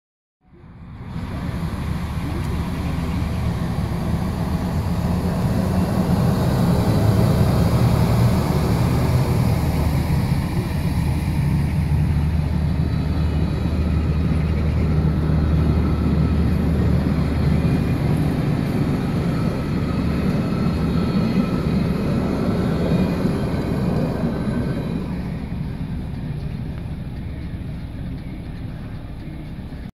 Öxnered og Ma 401 med omformer til Norge.
DDR omformer på vei til Norge og Bane Nor.